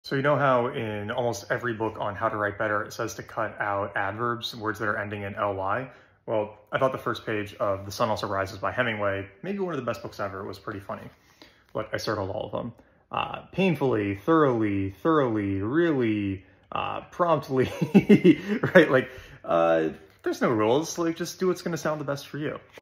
Don’t cut out Adverbs?
There are no rules! Maybe you don't have to delete those adverbs after all if you want to be a good writer.